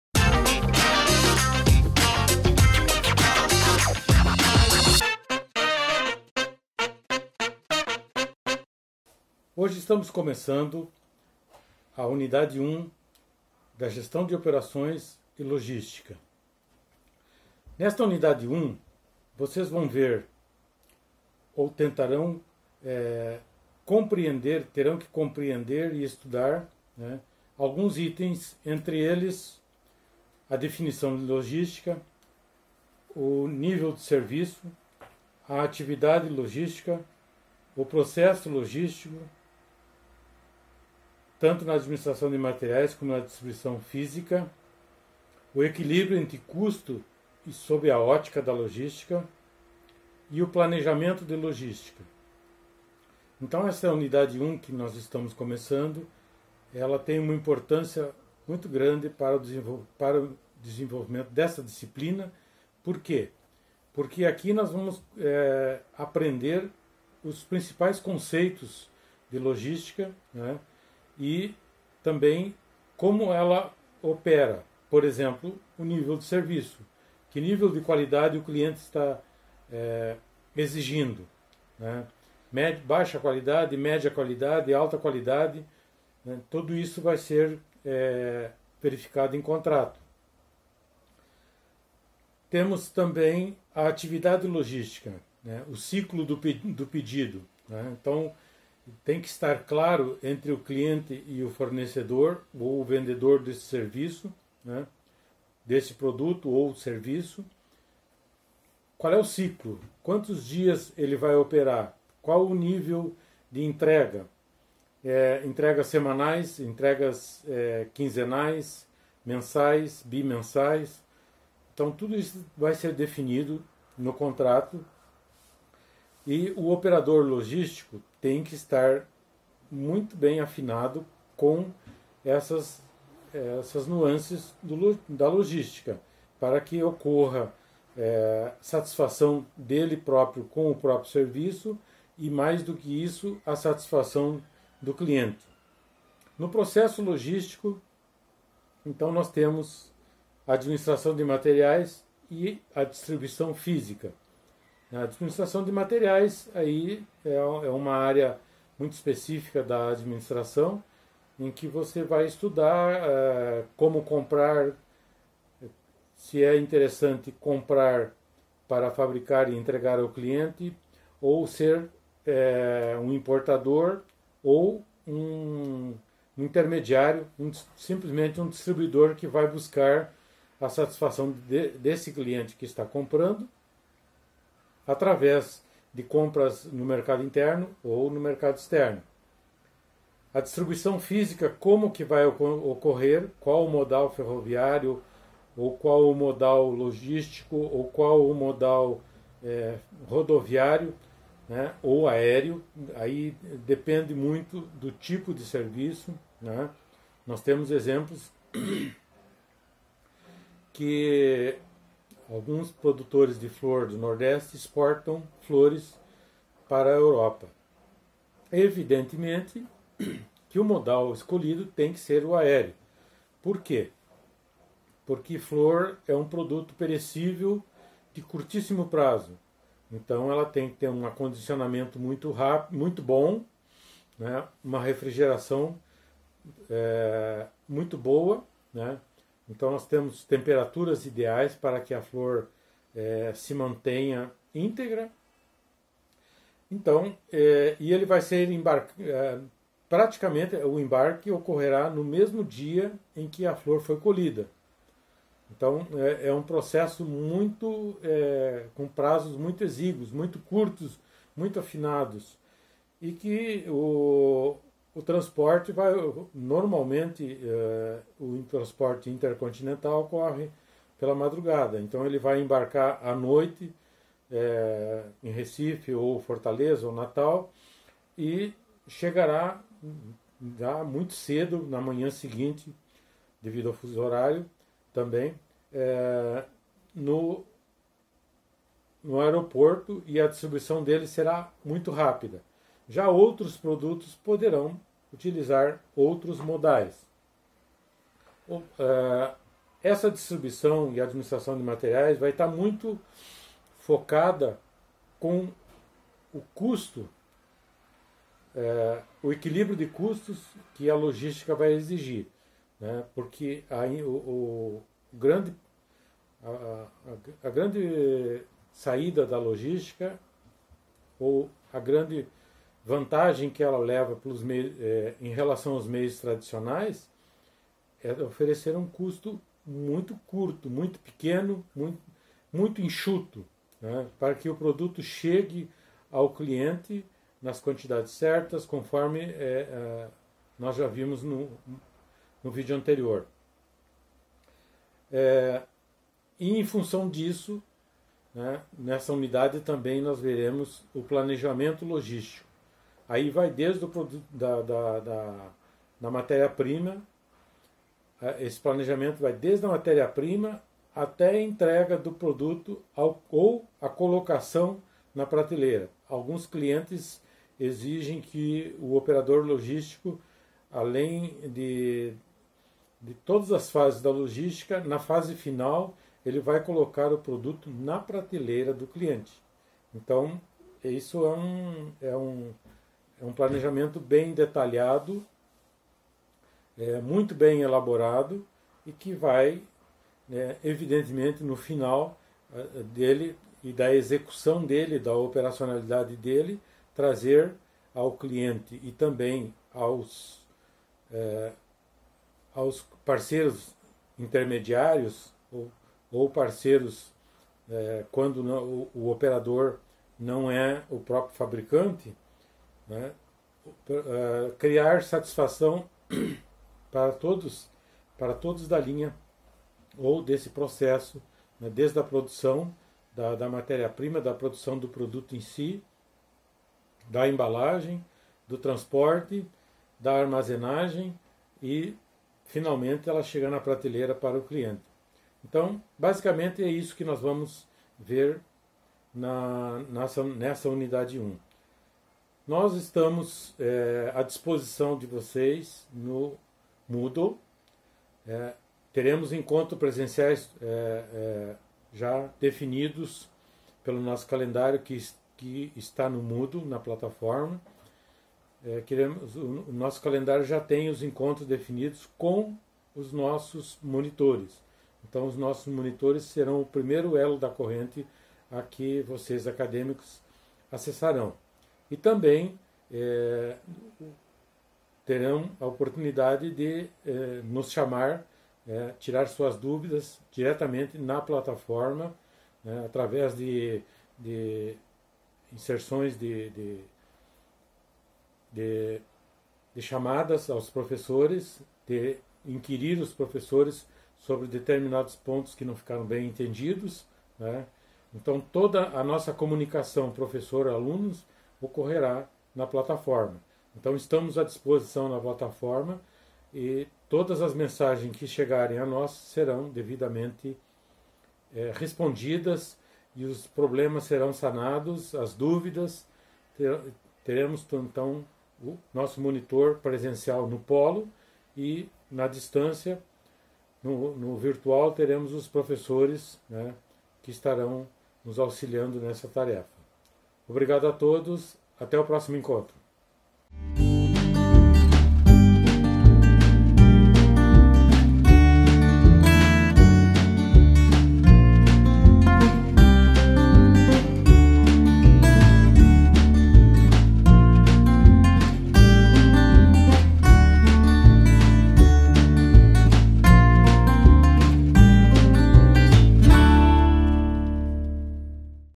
[ADMP] Introdução à Logística
Unidade 1 - Introdução à Logística para a disciplina Gestão de Operações e Logística I com o Professor Emiliano Elis Andrade Silva. Curso de Bacharelado em Administração Pública.